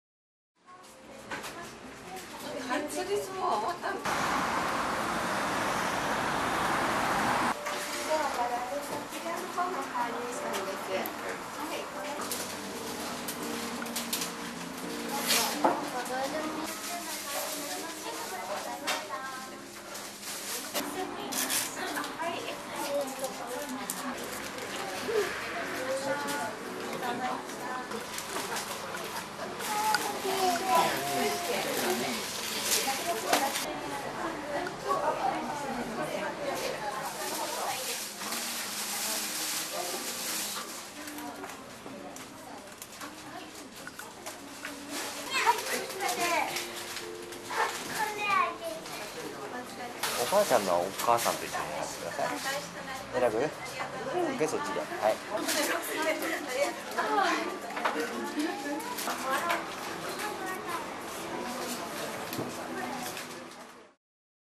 感謝の思い花に込め 「母の日」控え生花店にぎわう
１２日は「母の日」。秋田県内の生花店は１１日、母親へプレゼントする花を選ぶ人たちでにぎわった。